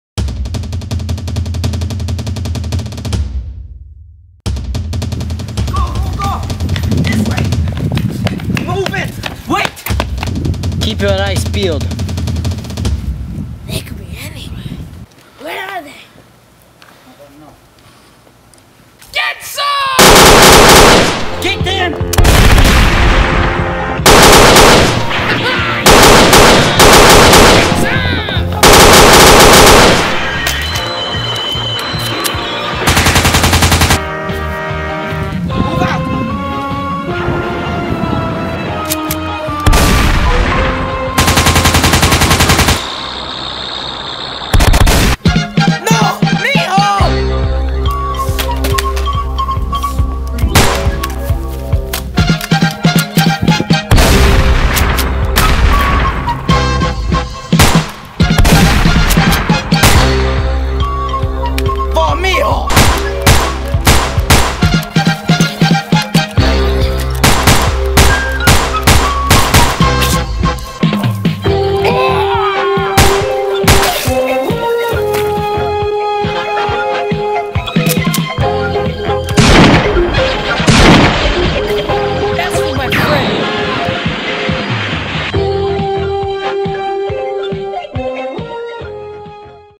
THE MILITIA: A Short Action Scene
a short action scene about militia men chasing some guy. Filmed on a Canon T2i.